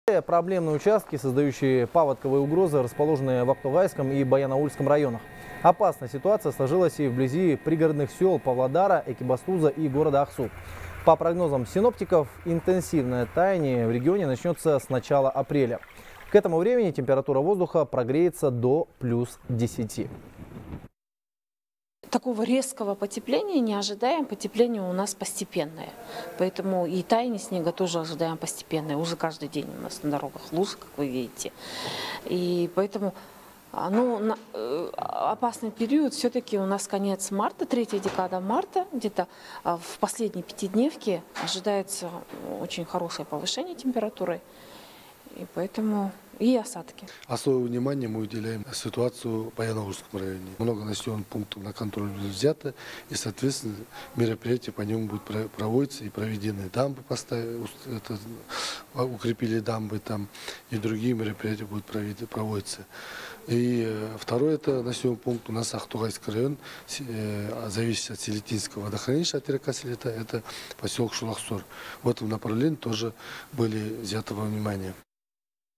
Опасная паводковая ситуация сложилась вблизи пригородных сёл Павлодара
Из населенных пунктов Павлодарской области вывезли больше миллиона кубометров снега. Но даже при этом количество снежной массы, скопившейся в сёлах, вызывает серьезные опасения в период весеннего половодья, сообщает «Хабар 24».